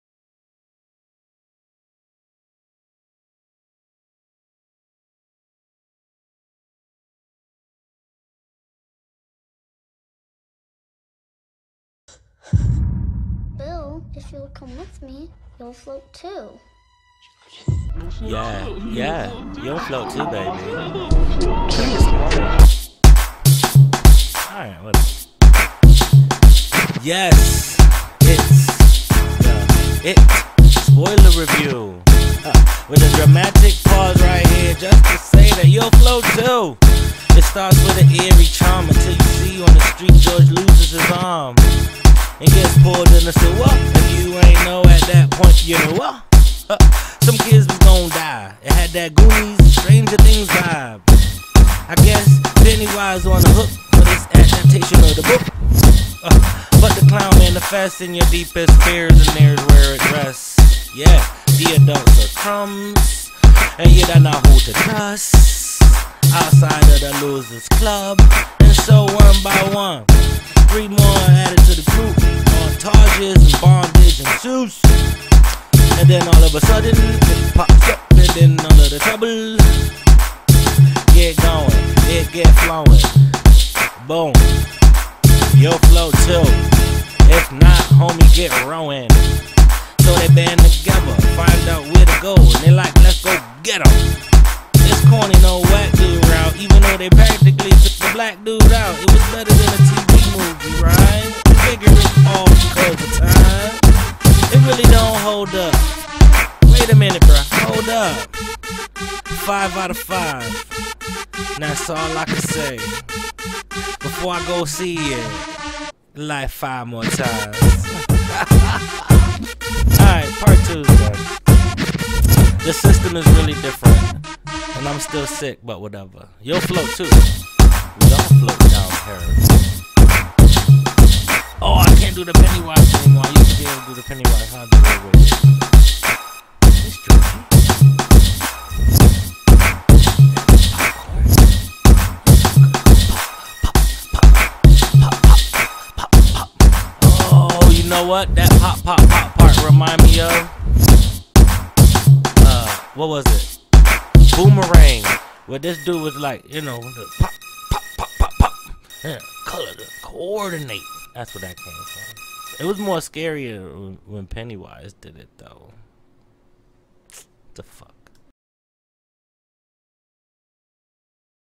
RAP REVIEWS: SEMI SPOILERS (IT) 2017 PT. 2 OF 3
This Is A Movie Review, In Hip Hop Form. Part 3 Drops When The Blu Ray Drops.